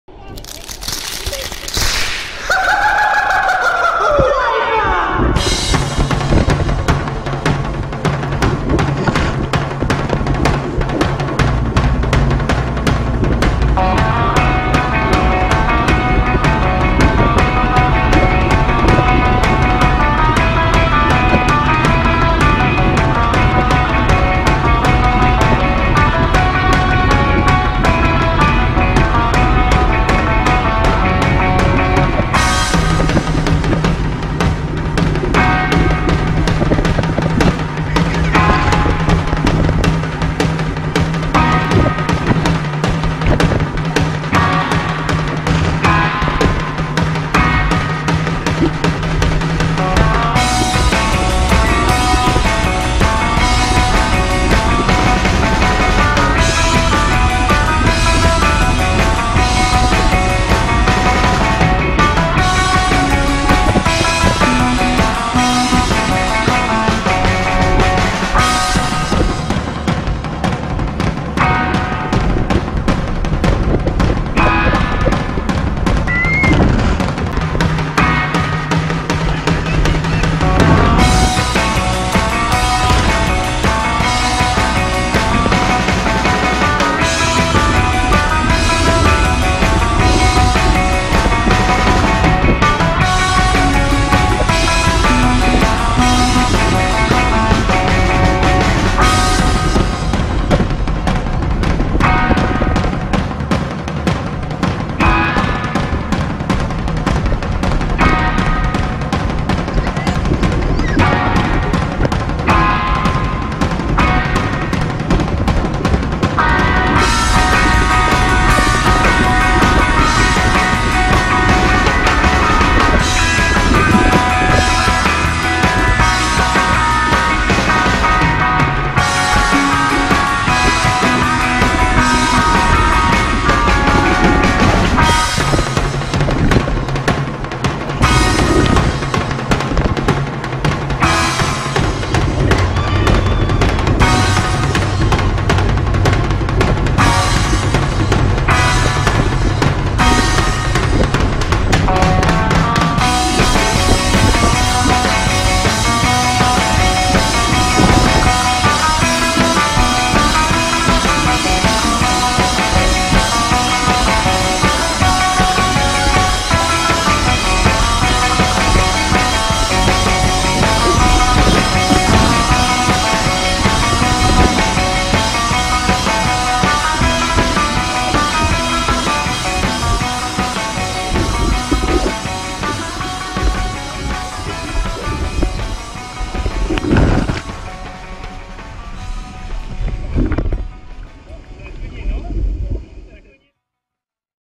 Session de BIG SUP avec LOCUSPORT 😁!!!
Des beaux fous-rires et de belles sensations pour cette session de BIG SUP !!!